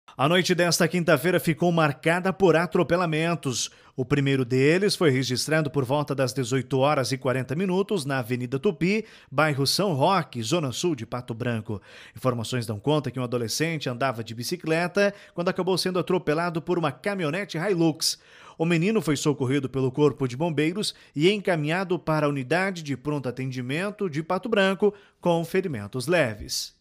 Atropelamento movimenta equipes de socorro na noite de quinta-feira (18) em Pato Branco
Segurança: Acidente do tipo atropelamento movimenta equipes de socorro na noite de quinta-feira (18) em Pato Branco.